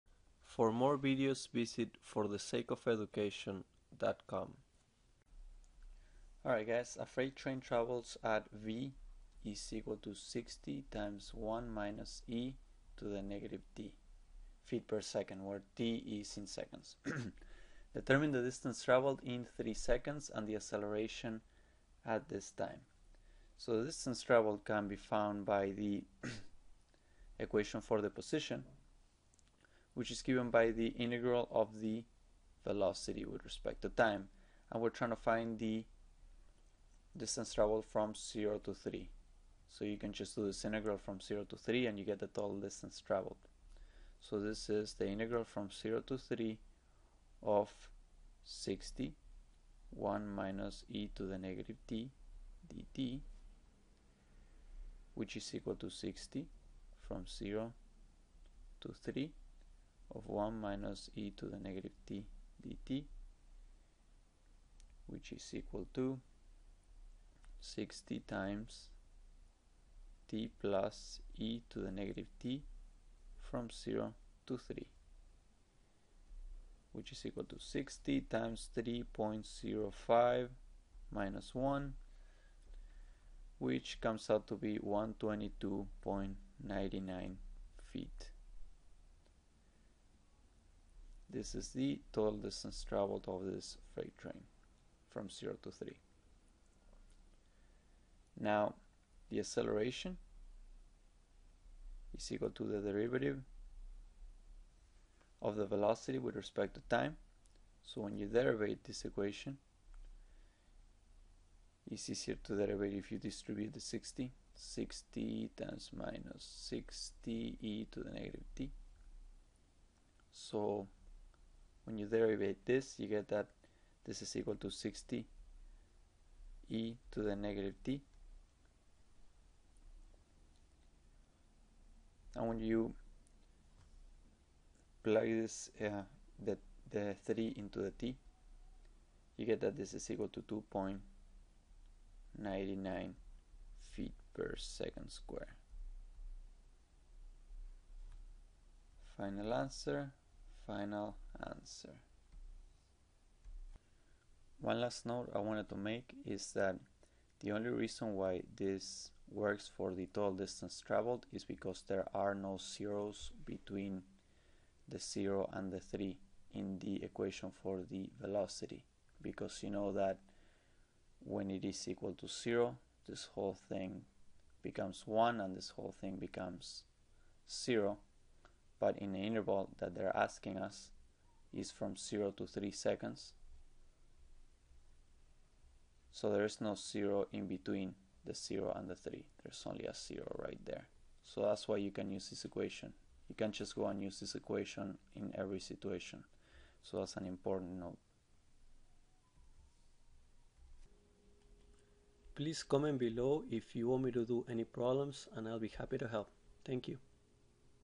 Determine the distance traveled in three seconds, and the acceleration at this time
A freight train travels at v = 60(1- e^( -t) ) ft/s,
where t is the elapsed time in seconds. Determine the
distance traveled in three seconds, and the acceleration at
this time.